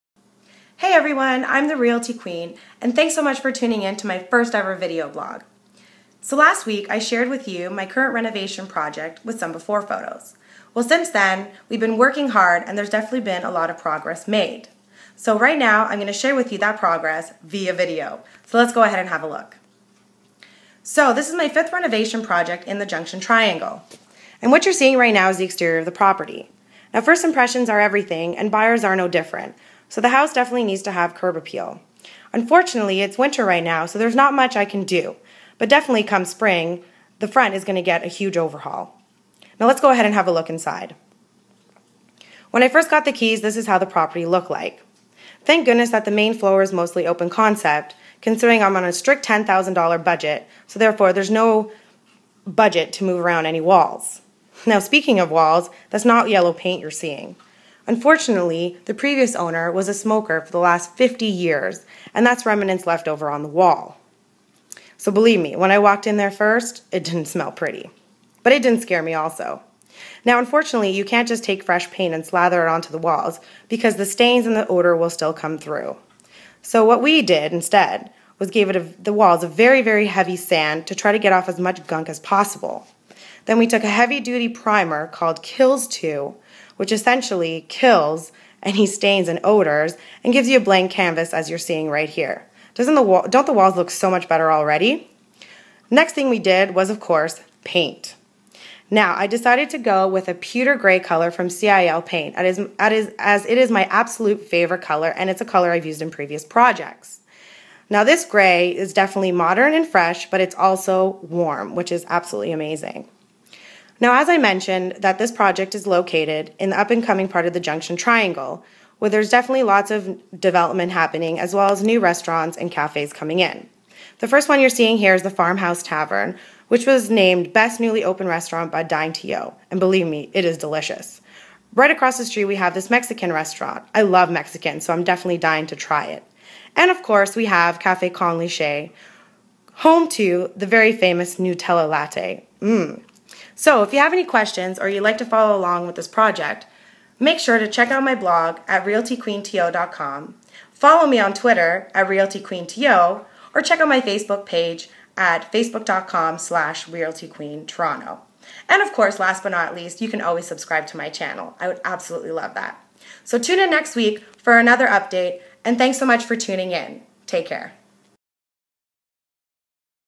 Renovation Update-The Paint
Join me on my journey as I renovate my 5th home in the Junction Triangle. Here is the first update, we've been busy priming and painting